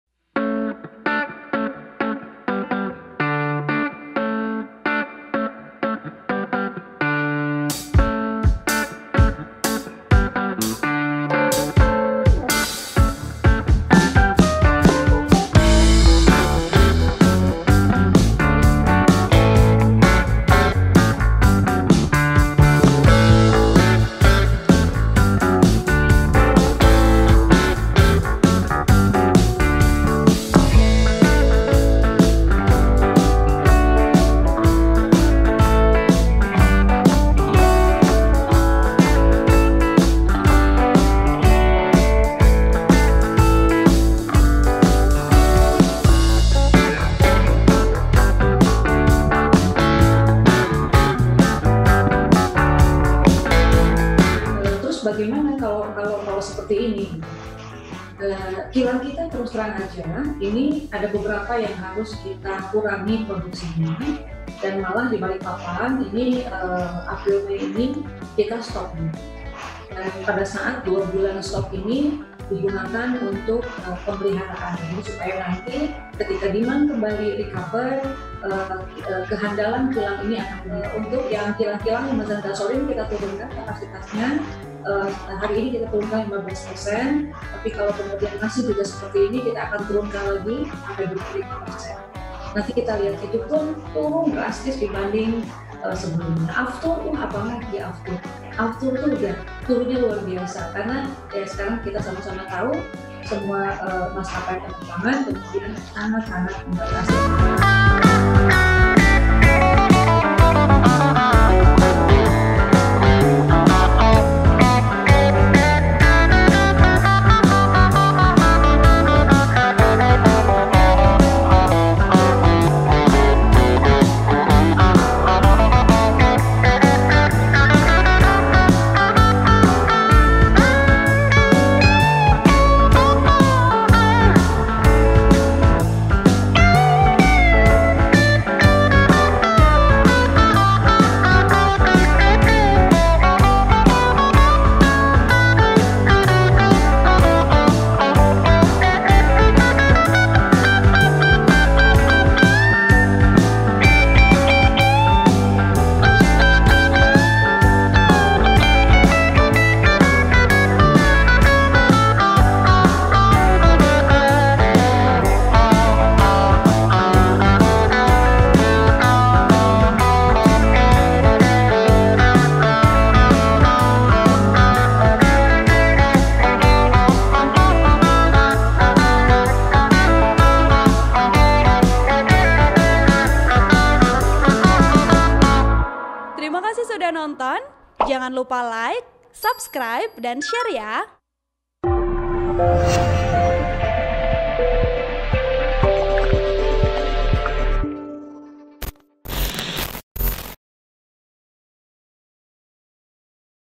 Penjualan Merosot Tajam, Pertamina Belum Turunkan Harga BBM, Nicke Akan Berikan Diskon
TRIBUN-VIDEO.COM - Pertama dalam sejarah, penjualan bahan bakar minyak atau BBM di Pertamina merosot sampai lebih dari 50 persen di sejumlah daerah, terutama DKI Jakarta.

Tak hanya terjadi di ibu kota, penurunan penjualan BBM lebih dari 50 persen juga terjadi di kota-kota besar lainnya seperti Medan, Makassar, Surabaya, dan Bandung.

Jaga jarak sosial sampai fisik berpengaruh besar pada konsumsi bensin. Dan secara nasional, bensin sudah ambles 25 persen.

Sepi pembeli tak lantas membuat Pertamina mengobral harga BBM.

Baca Juga: Gimana Cara Bedakan Flu Gejala Corona dengan Flu Alergi Dingin atau Debu?

Belum ada rencana penurunan harga bahan bakar minyak atau BBM yang menyesuaikan rendahnya harga minyak mentah dunia.

Mengapa? Karena 93 persen belanja modal sekaligus operasional Pertamina menggunakan mata uang dollar.

Padahal, kurs dollar saat ini sedang mahal terhadap rupiah. Tapi yang jelas, sejumlah kilang Pertamina terpaksa ditutup.

Editor : Christandi Dimas